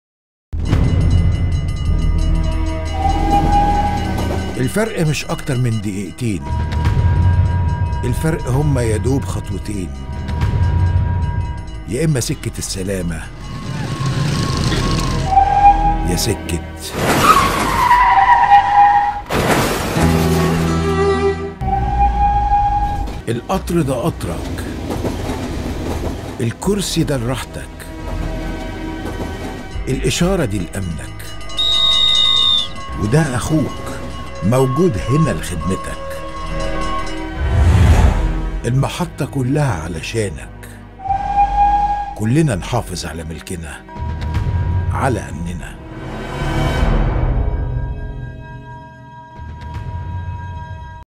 حافظ على حياتك وحياة أولادك.. "النقل" تناشد المواطنين بعدم اقتحام مزلقانات القطارات أثناء غلقها
ناشدت  وزارة النقل السادة المواطنين بضرورة توخي الحذر عند عبور مزلقانات السكة الحديد والالتزام بتعليمات الهيئة عند العبور والالتزام بالعبور بعد فتح المزلقان ومرور القطار وعدم  اقتحام المزلقان او السير عكس الاتجاه أثناء غلقه حتى لايؤدي ذلك إلى ازهاق الارواح وتعطيل مسير القطارات